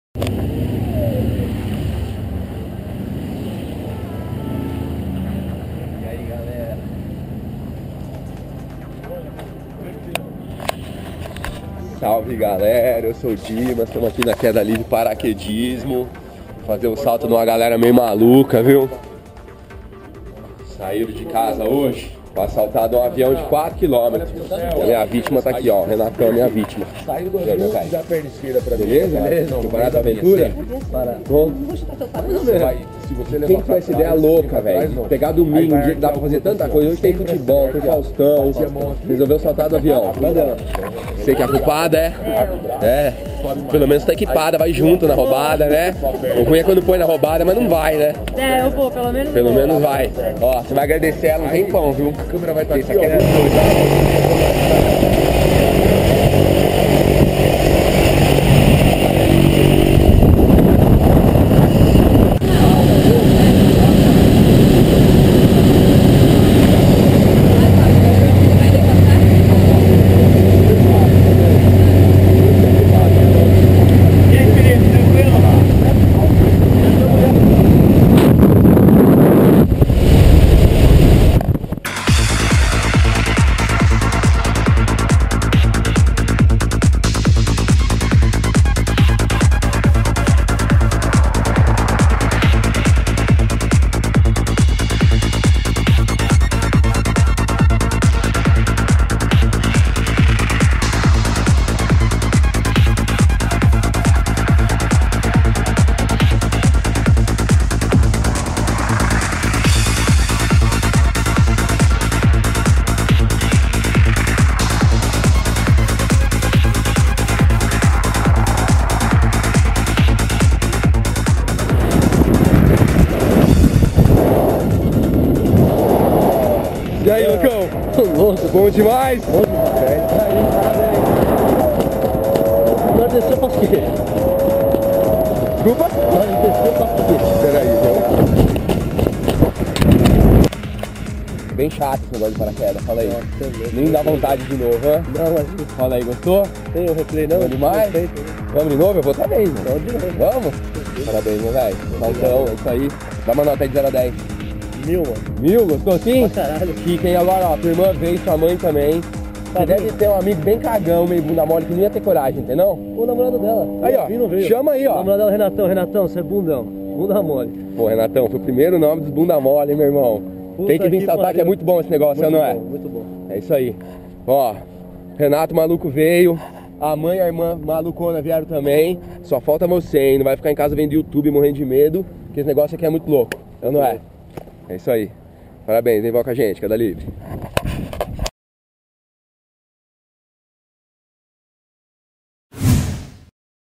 Salto de Paraquedas do Renato na Queda Livre Paraquedismo
Data: 15/07/2012

Passageiro: Renato

Instrutor: Dimas

Escola: Queda Livre Paraquedismo